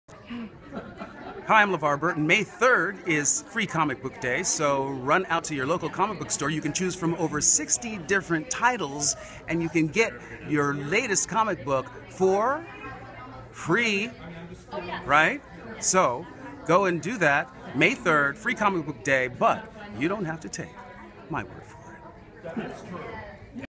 LeVar Burton Announces Free Comic Book Day
A very special meet and greet opportunity was a prize I won at the Disney Social Media Moms Celebration in Disneyland where I met LeVar Burton and I recorded a special message from him to share with you. May 3rd is Free Comic Book day with 60 titles to choose from so every child can find a story they like to read.